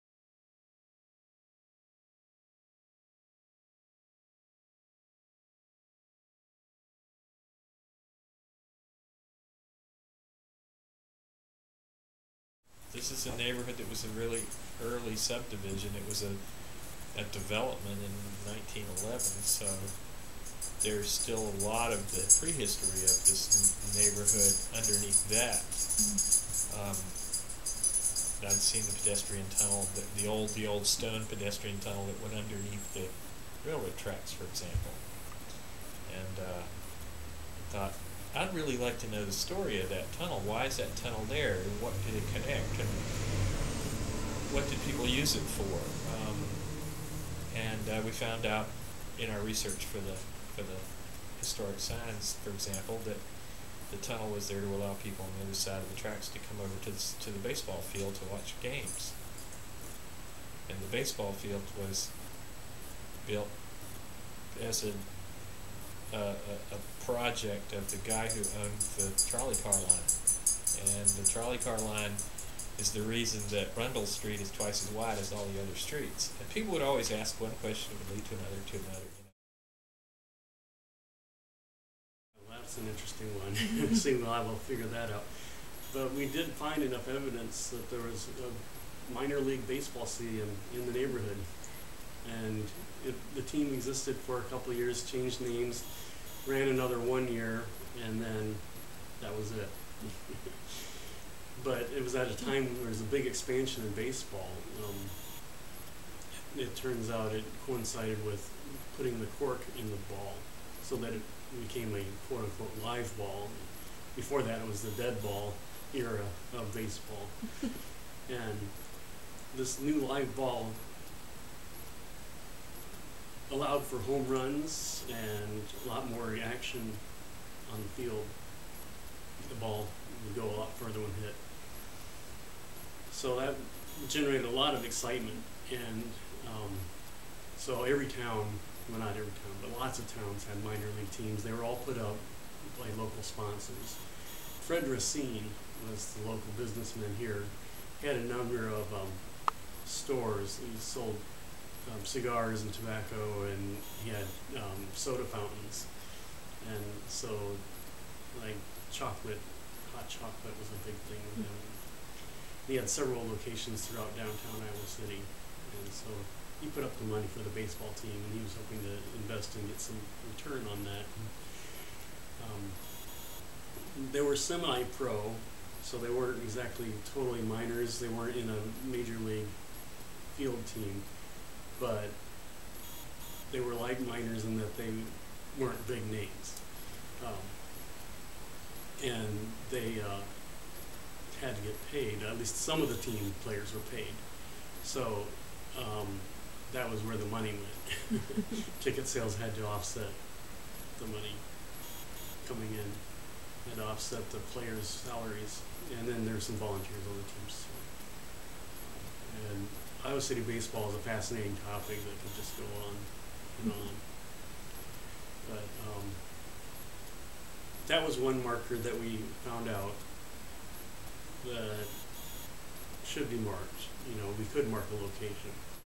The Pedestrian Tunnel and the Rundell Baseball Park
During interviews conducted in November 2013, Will Thomson and Tim Weitzel talk about the Pedestrian Tunnel and the Rundell Baseball Park in the Longfellow Neighborhood of Iowa City.